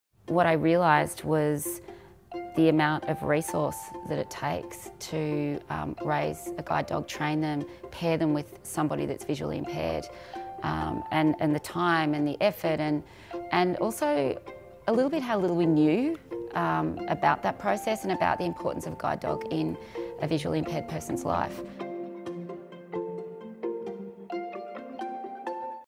Gorman x Guide Dogs 2018 - Breeding Raising and Training a Guide Dog
Lisa Gorman explains why Gorman decided to partner with Guide Dogs Victoria again in 2018